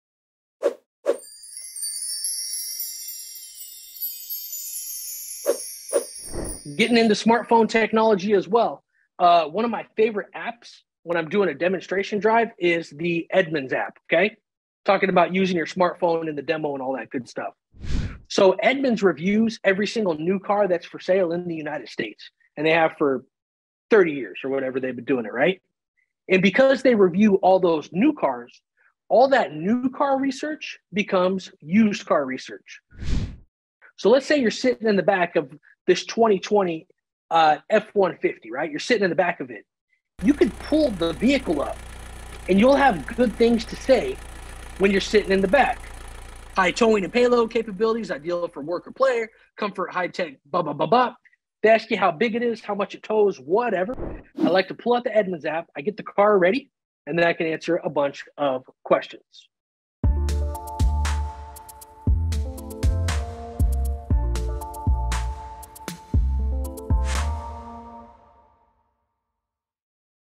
My Favorite App for Reviews
Want to be the car guru during your next demo drive?

For 30 years, Edmonds has been our go-to for reviews on every new car in the U.S. 🇺🇸 But here's the kicker – all that new car intel transforms into used car gold! So, whether you're a car enthusiast or just curious, this app is a game-changer.

Pull up the vehicle on Edmonds, and suddenly you're a fountain of knowledge. 💡 High towing and payload capabilities? Check. Ideal for work or play? Double-check. High-tech features that'll make heads turn? You got it! 🤯

Especially when dealing with a used car you might not know well, the Edmonds app is my trusty sidekick. 🦸‍♂️